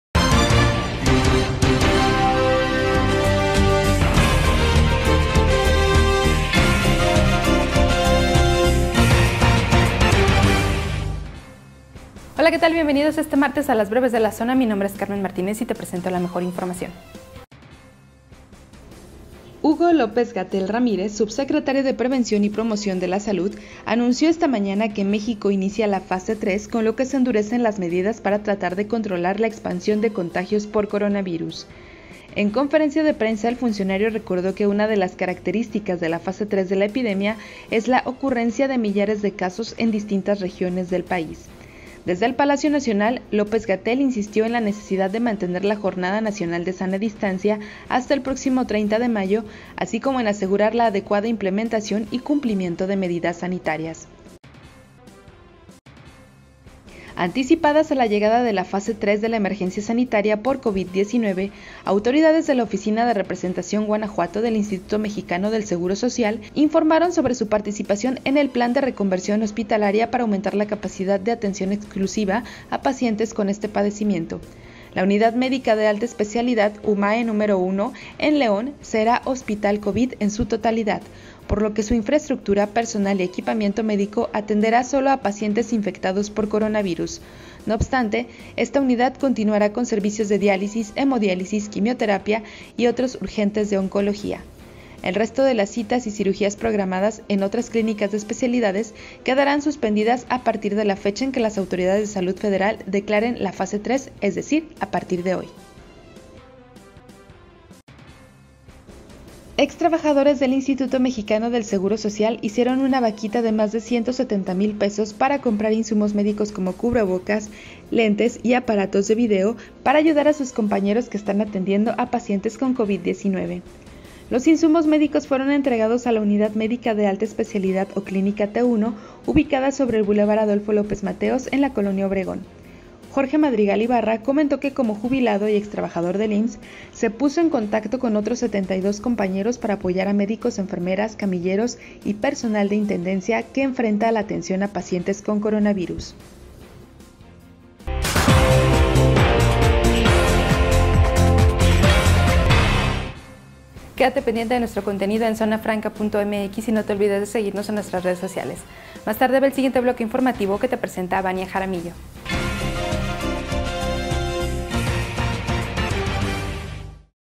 El Gobierno anuncia que México entró hoy a la Fase 3 | Breves de la Zona
BrevesDeLaZona📰 | Esto es LoMásDestacado de esta tarde
- Pensionados del IMSS apoyan con insumos para atender a pacientes con COVID19.
- El Gobierno anuncia que México entró hoy a la Fase 3. La epidemia “está en ascenso”: López-Gatell.
- Atención en UMAE No.1 del IMSS será exclusiva para pacientes con COVID19.